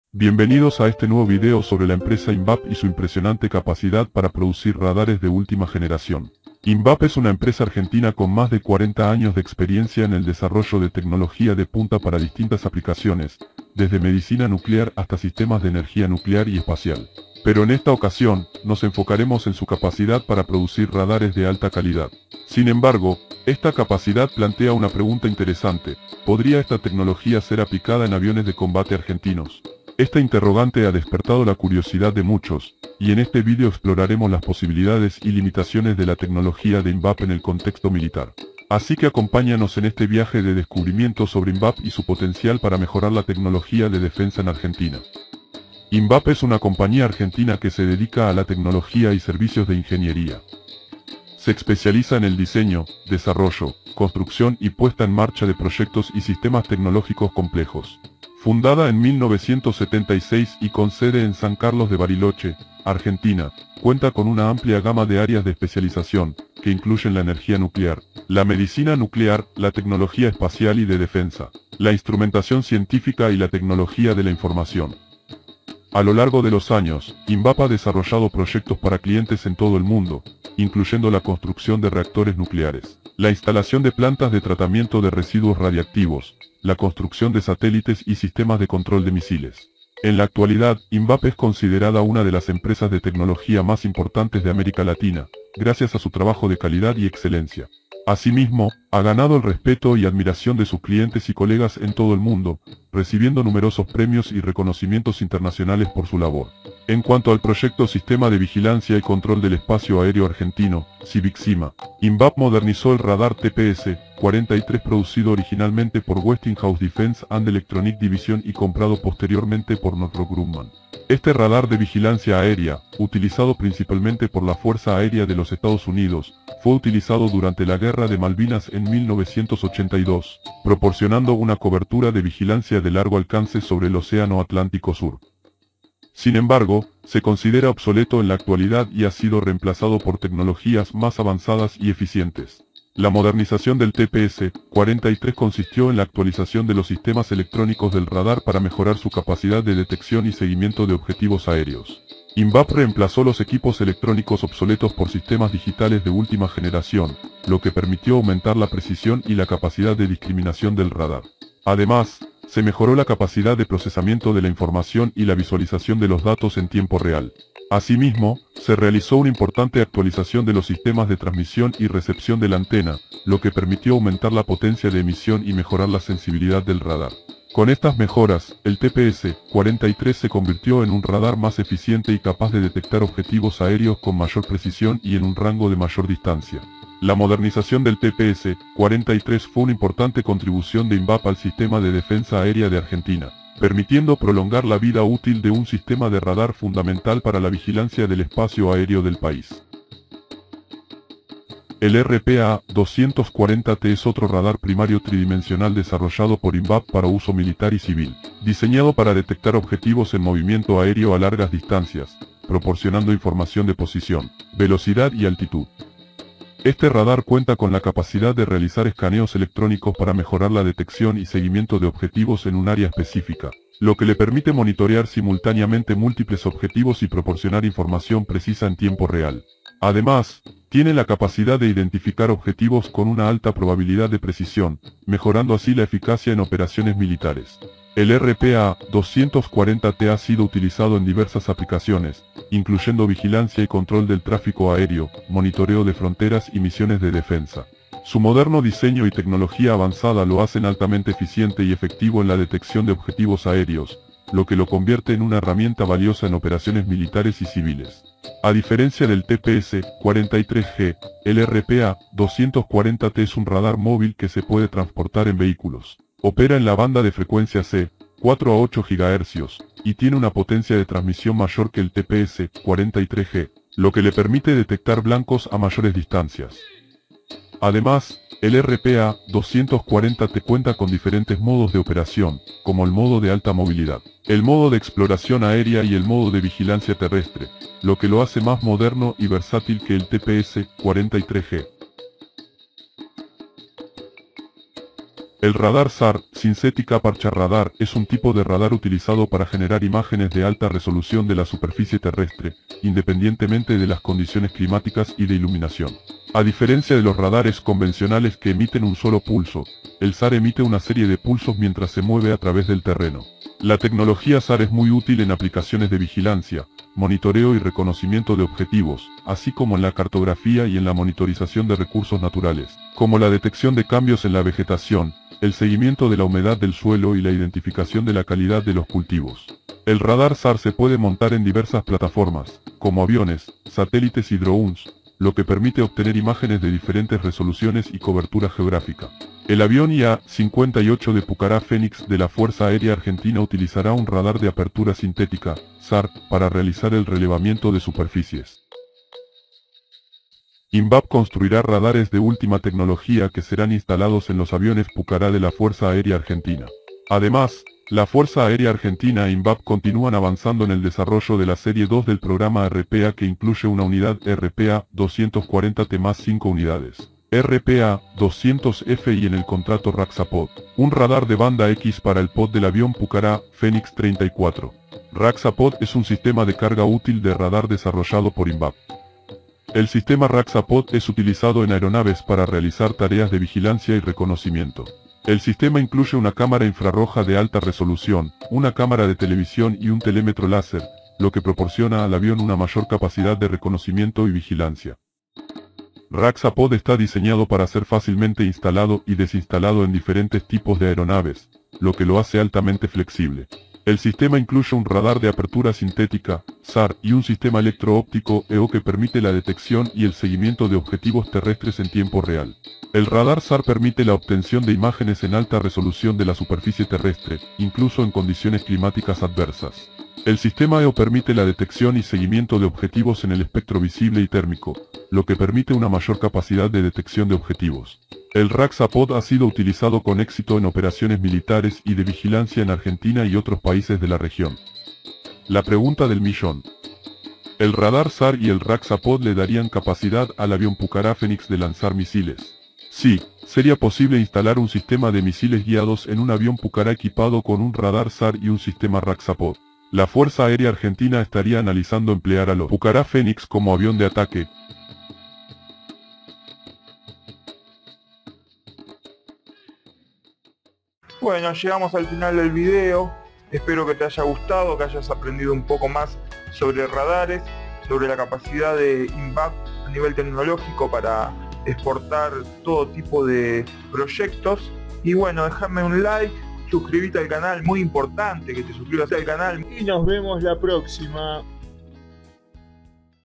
INVAP, RADARES Argentinos de última Generación | Exportación De Tecnología
INVAP es una compañía argentina que se dedica a la tecnología y servicios de ingeniería. Se especializa en el diseño, desarrollo, construcción y puesta en marcha de proyectos y sistemas tecnológicos complejos. Fundada en 1976 y con sede en San Carlos de Bariloche, Argentina, cuenta con una amplia gama de áreas de especialización, que incluyen la energía nuclear, la medicina nuclear, la tecnología espacial y de defensa, la instrumentación científica y la tecnología de la información. A lo largo de los años, INVAP ha desarrollado proyectos para clientes en todo el mundo, incluyendo la construcción de reactores nucleares, la instalación de plantas de tratamiento de residuos radiactivos, la construcción de satélites y sistemas de control de misiles.

En la actualidad, INVAP es considerada una de las empresas de tecnología más importantes de América Latina, gracias a su trabajo de calidad y excelencia. Asimismo, ha ganado el respeto y admiración de sus clientes y colegas en todo el mundo, recibiendo numerosos premios y reconocimientos internacionales por su labor.

En cuanto al proyecto "Sistema de Vigilancia y Control del Espacio Aéreo Argentino" (SIVICCIMA), INVAP modernizó el radar TPS-43 producido originalmente por Westinghouse Defense and Electronic Division y comprado posteriormente por Northrop-Grumman. Este radar de vigilancia aérea, utilizado principalmente por la Fuerza Aérea de los Estados Unidos, fue utilizado durante la Guerra de Malvinas en 1982, proporcionando una cobertura de vigilancia de largo alcance sobre el Océano Atlántico Sur. Sin embargo, se considera obsoleto en la actualidad y ha sido reemplazado por tecnologías más avanzadas y eficientes.

La modernización del TPS-43 consistió en la actualización de los sistemas electrónicos del radar para mejorar su capacidad de detección y seguimiento de objetivos aéreos. INVAP reemplazó los equipos electrónicos obsoletos por sistemas digitales de última generación, lo que permitió aumentar la precisión y la capacidad de discriminación del radar. Además, se mejoró la capacidad de procesamiento de la información y la visualización de los datos en tiempo real.

Asimismo, se realizó una importante actualización de los sistemas de transmisión y recepción de la antena, lo que permitió aumentar la potencia de emisión y mejorar la sensibilidad del radar. Con estas mejoras, el TPS-43 se convirtió en un radar más eficiente y capaz de detectar objetivos aéreos con mayor precisión y en un rango de mayor distancia.

La modernización del TPS-43 fue una importante contribución de INVAP al sistema de defensa aérea de Argentina, permitiendo prolongar la vida útil de un sistema de radar fundamental para la vigilancia del espacio aéreo del país.

El RPA-240T es otro radar primario tridimensional desarrollado por INVAP para uso militar y civil, diseñado para detectar objetivos en movimiento aéreo a largas distancias, proporcionando información de posición, velocidad y altitud. Este radar cuenta con la capacidad de realizar escaneos electrónicos para mejorar la detección y seguimiento de objetivos en un área específica, lo que le permite monitorear simultáneamente múltiples objetivos y proporcionar información precisa en tiempo real. Además, tiene la capacidad de identificar objetivos con una alta probabilidad de precisión, mejorando así la eficacia en operaciones militares.

El RPA-240T ha sido utilizado en diversas aplicaciones, incluyendo vigilancia y control del tráfico aéreo, monitoreo de fronteras y misiones de defensa. Su moderno diseño y tecnología avanzada lo hacen altamente eficiente y efectivo en la detección de objetivos aéreos, lo que lo convierte en una herramienta valiosa en operaciones militares y civiles.

El radar SAR (Synthetic Aperture Radar) es un tipo de radar utilizado para generar imágenes de alta resolución de la superficie terrestre, independientemente de las condiciones climáticas y de iluminación. A diferencia de los radáres convencionales que emiten un solo pulso, el SAR emite una serie de pulsos mientras se mueve a través del terreno.

RAXA-POD es un sistema de carga útil de radar desarrollado por INVAP. El sistema RAXA-POD es utilizado en aeronaves para realizar tareas de vigilancia y reconocimiento. El sistema incluye una cámara infrarroja de alta resolución, una cámara de televisión y un telémetro láser, lo que proporciona al avión una mayor capacidad de reconocimiento y vigilancia.